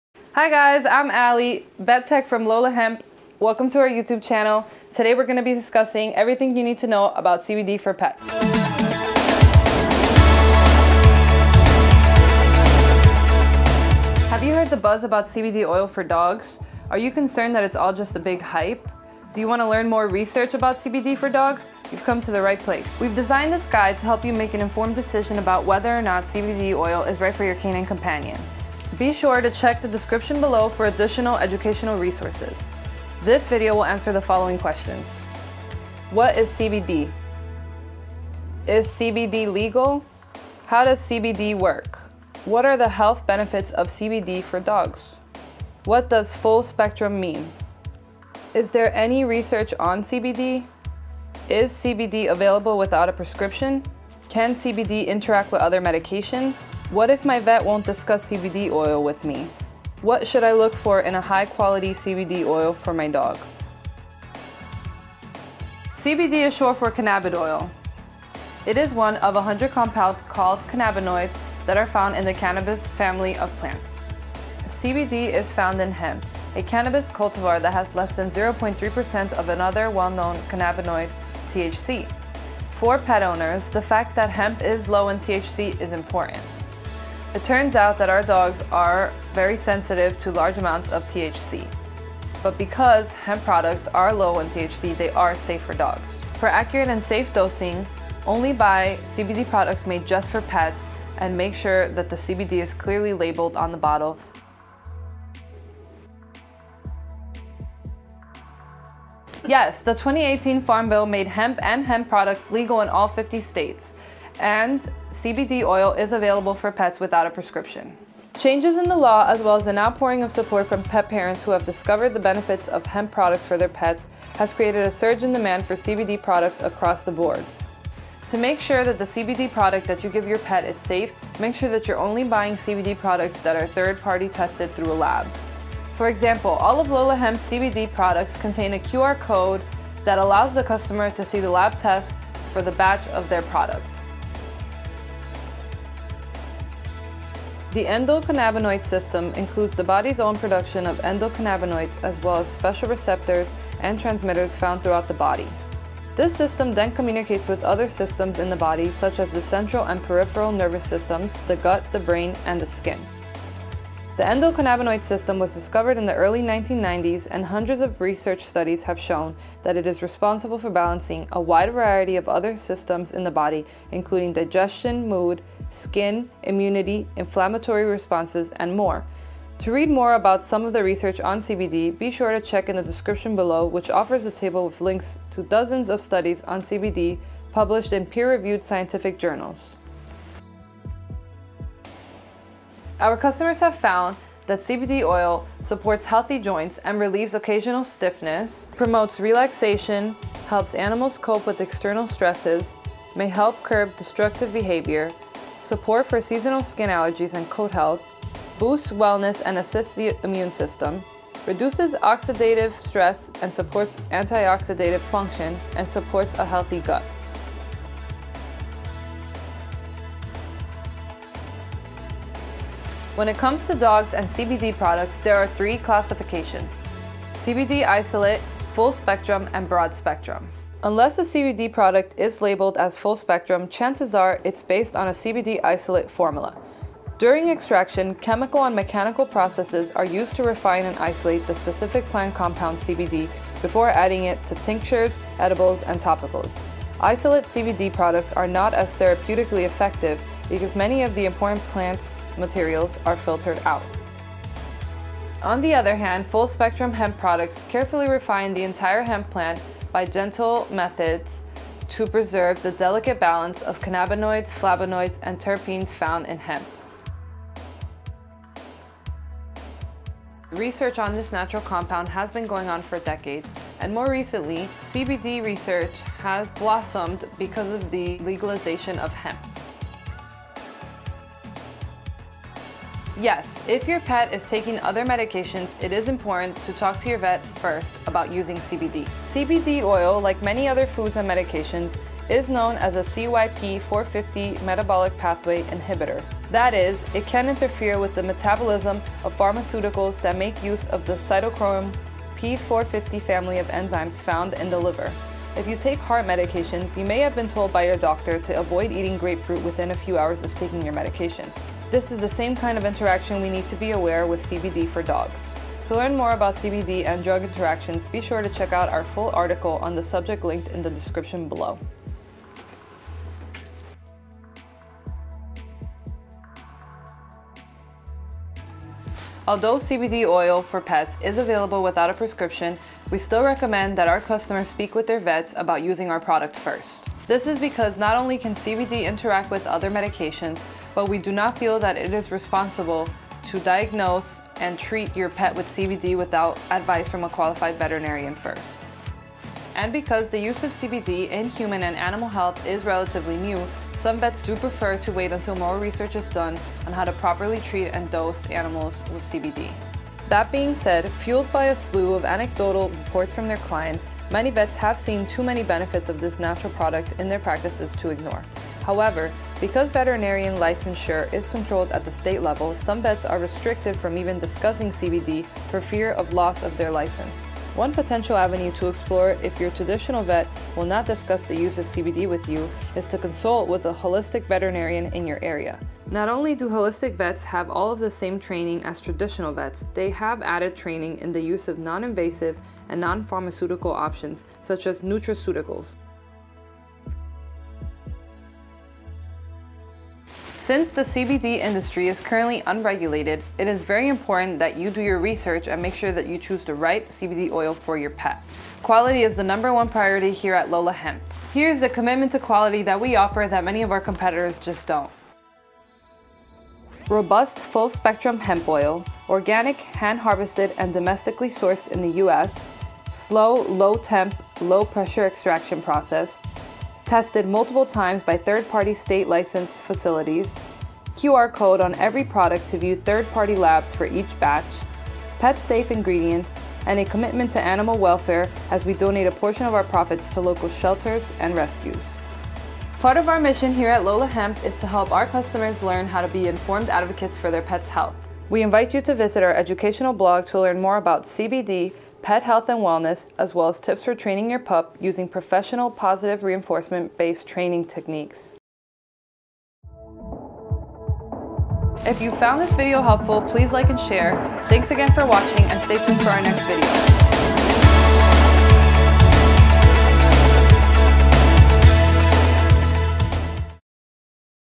CBD Oil for Dogs: Everything You Need to Know
Do you want to learn more about CBD Oil for Dogs? This video will answer the most commonly asked questions about CBD oil for dogs.

For more information about CBD for dogs as well as a list of links to scientific research on CBD

View our other videos related to CBD and Pets